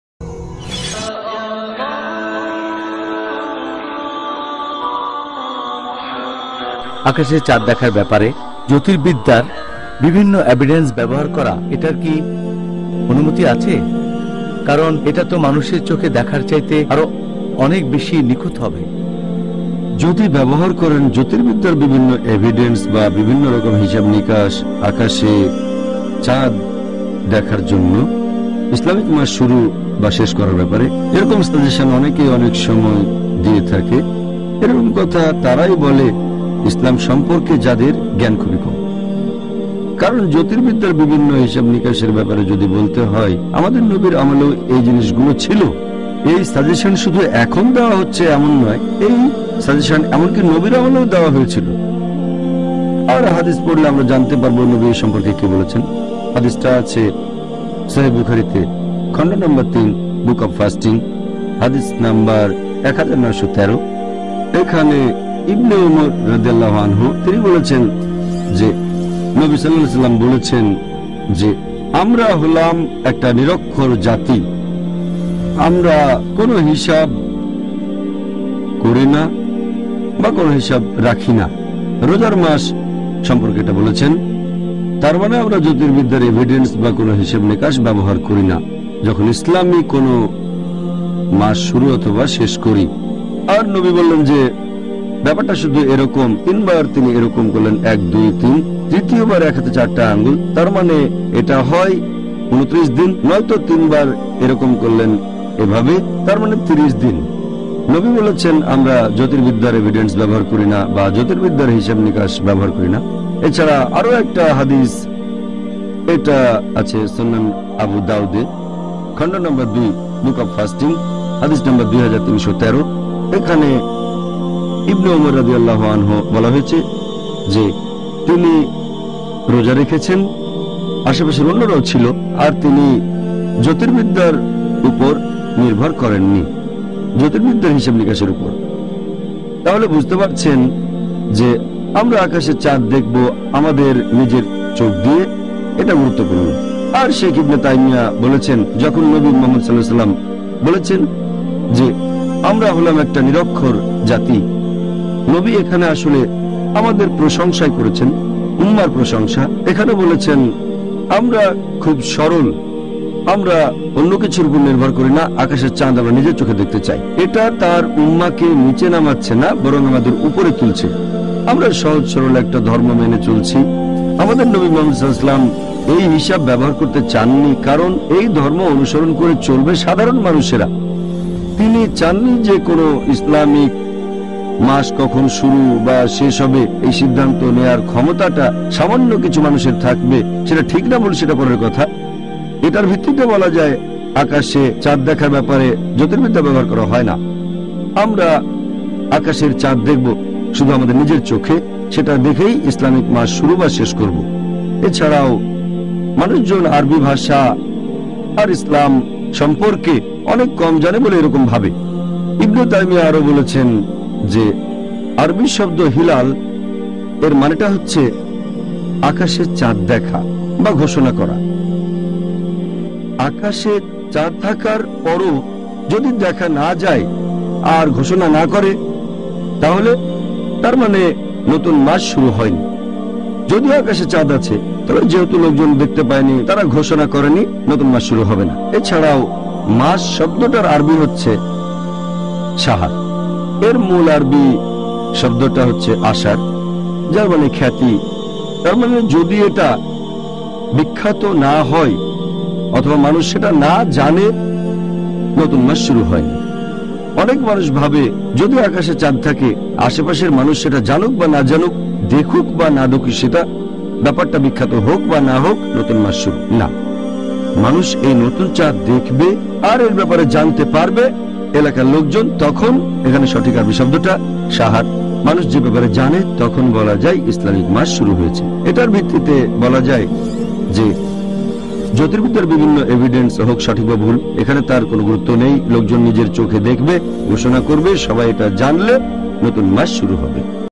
আকাশে চাঁদ দেখার জন্য জ্যোতিরবিদ্যা ব্যবহার করা যাবে?
Answered by dr jakir naik

উত্তর দিচ্ছেন ডাঃজাকির নায়েক

Full Audio Lecture

 Ramdan date with Dr zakir Bangla

⏭️ বিভিন্ন বিষয় নিয়ে প্রশ্ন ও উত্তর আলোচনা করেছেন
ডাঃ জাকির নায়েক
ভিডিওটি শেয়ার করতে পারেন
আরো জানতে চাইলে দেখুন
প্রশ্ন ও উত্তর
🔀 Ramdan date with Dr zakir
আমাদের ফেসবুক পেজ লাইক করতে পারেন
ধন্যবাদ 🔝